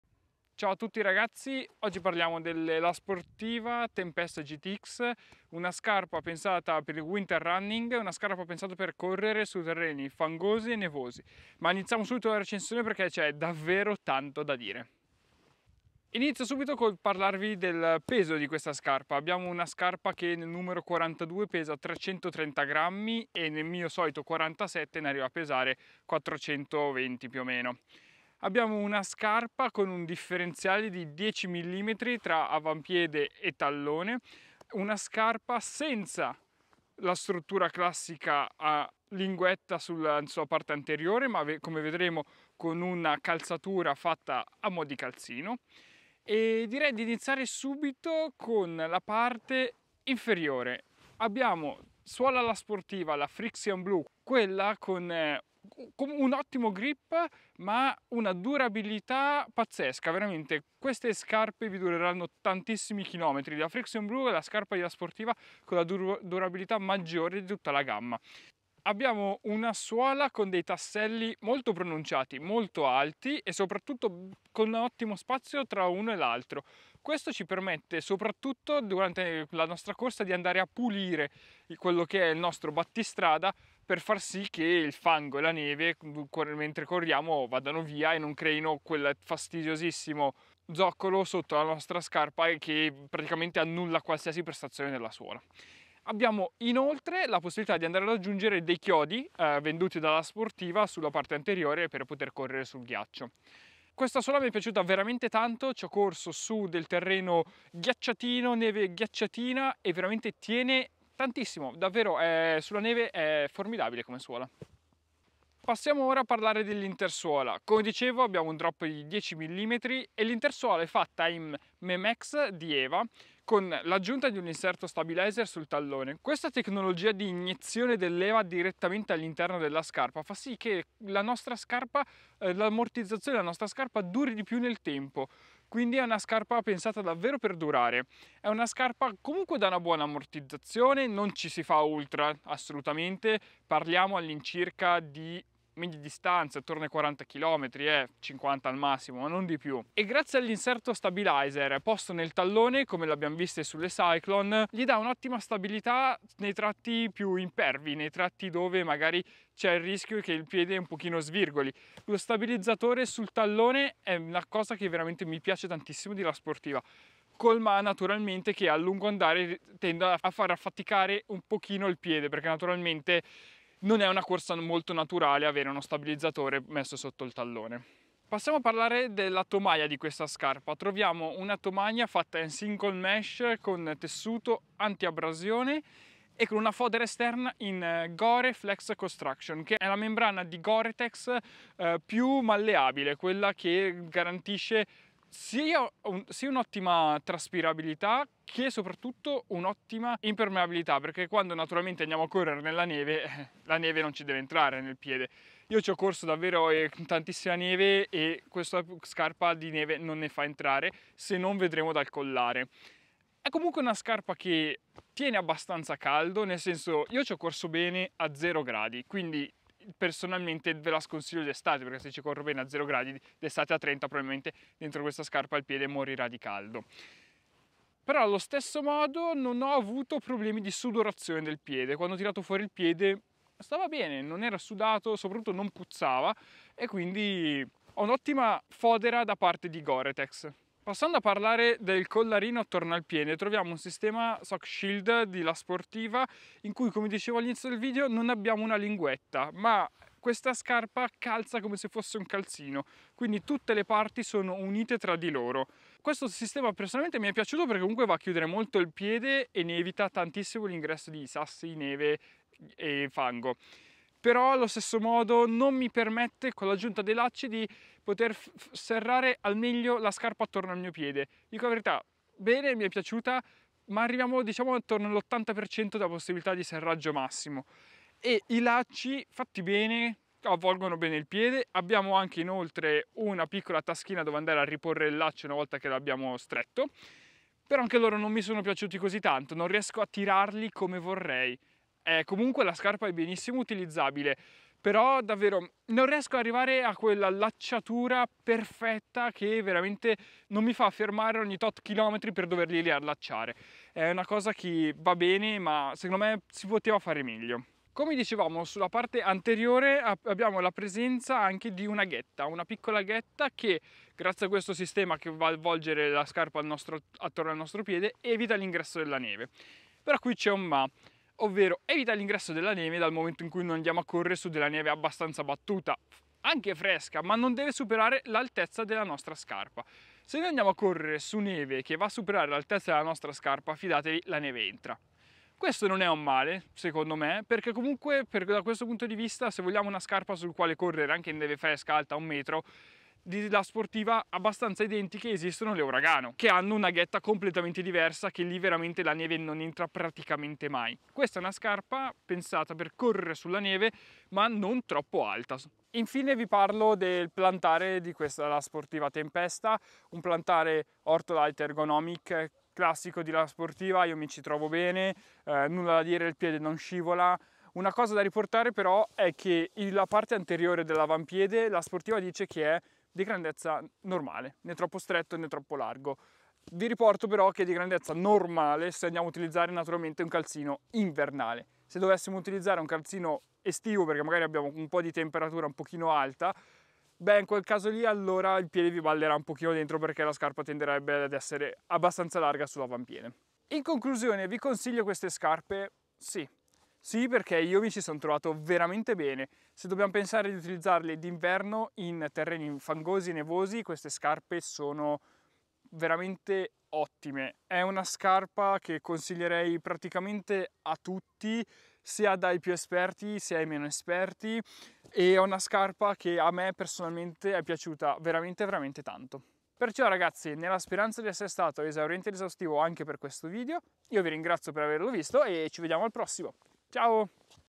PERFETTE per la neve (ed ECONOMICHE) - Recensione La Sportiva Tempesta GTX
La Sportiva Tempesta GTX sono le scarpe da trail running ideate per correre sulla neve, sul guiaccio ed anche a zero gradi.

Queste scarpe mi hanno accompagnato nei miei allenamenti sulla neve restituendomi ottime performance grazie al membrana in Goretex e alla suola Frixion di La Sportiva.

Se siete interessati a delle scarpe per i vostri allenamenti invernali e cercate una scarpa adatta anche a terreni scivolosi o ghiacciati, questo è il video che fa per voi.
Buona visione!

🛒 Link per l'acquisto

CAPITOLI
00:00 Introduzione
00:16 Caratteristiche
00:50 Suola Frixion Blu
01:55 Intersuola
02:58 Tomaia
04:01 Sistema Sock-Shield e allacciatura
05:07 Ghetta
05:56 Plantare
06:34 Considerazioni finali
07:00 Conclusioni

P.S.